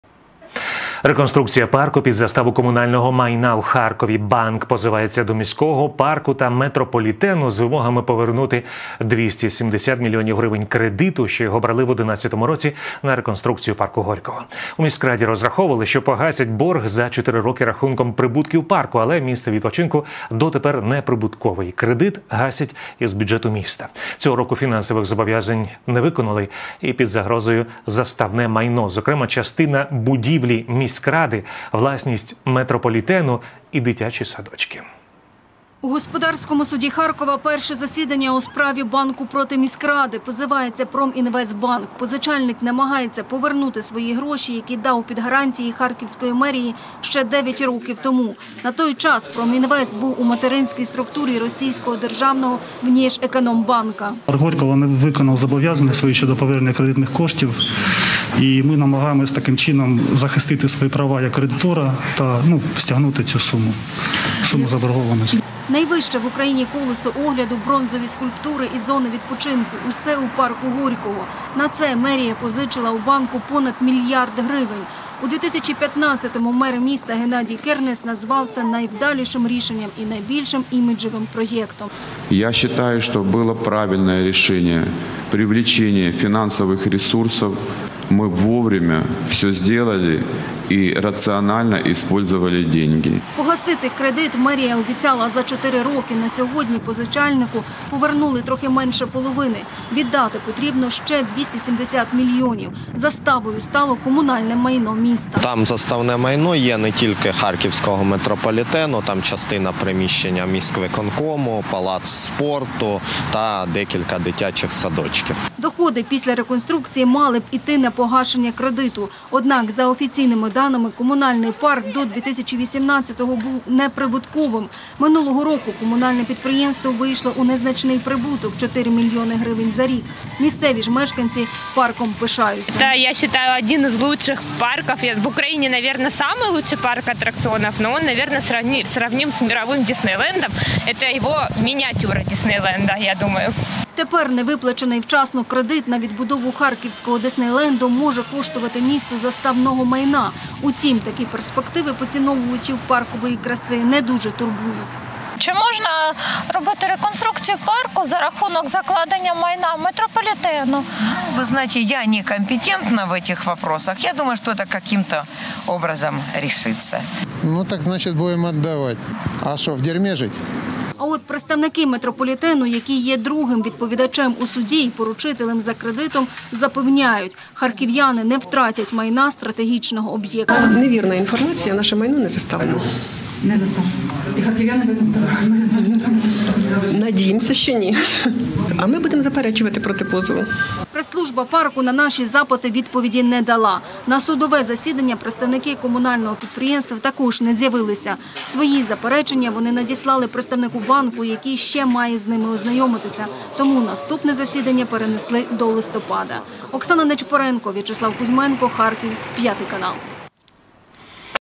Метрополітен під заставою: банк позивається до міської влади Харкова з вимогами повернути 270 млн
Реконструкція парку під заставу комунального майна. У Харкові банк позивається до міського парку та метрополітену з вимогами повернути 270 мільйонів гривень кредиту, що його брали в 11 році на реконсрукцію парку Горького. У Міськраді розраховували, що погасять борг за 4-ри роки рахунком прибутків парку. Але місце відпочинку дотепер неприбуткове, і кредит гасять із бюджету міста. Цього року фінансових зобов’язань не виконали і під загрозою заставне майно: зокрема частина будівлі міськради, власність метрополітену і дитячі садочки.